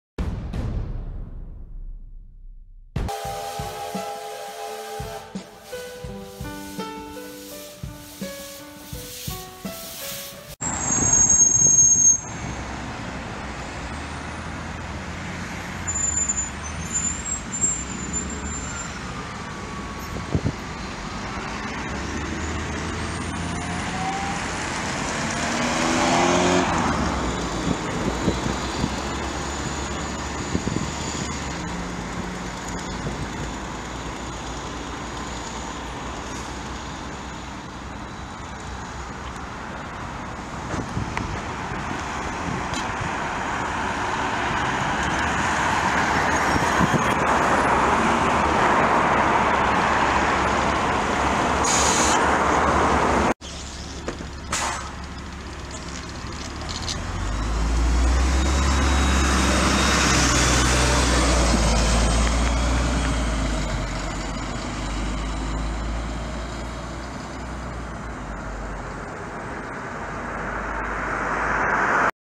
[D3500] Микро-автобус "ГАЗель City" ГАЗ-А68R52 №37 и автобус ЛиАЗ-5292.65 №5497
Микро-автобус "ГАЗель City" ГАЗ-А68R52 №37 (Приписан и обслуживается ООО "АвтоЛайн")
Следует в парк.

Пр.Народного Ополчения.

Автобус ЛиАЗ-5292.65 №5497 (Приписан и обслуживается ГУП "Пассажиравтотранс",Автобусный парк №5)
Маршрут №229 -
"Кировский завод - Ж.д.о.п."Сергиево"".

Прибывает на ост."Ул.Партизана Германа",отправляется с ост."Ул.Пограничника Гарькавого,43",Пр.Народного Ополчения,Ул.Пограничника Гарькавого,Санкт-Петербург,Россия.
10.III.2021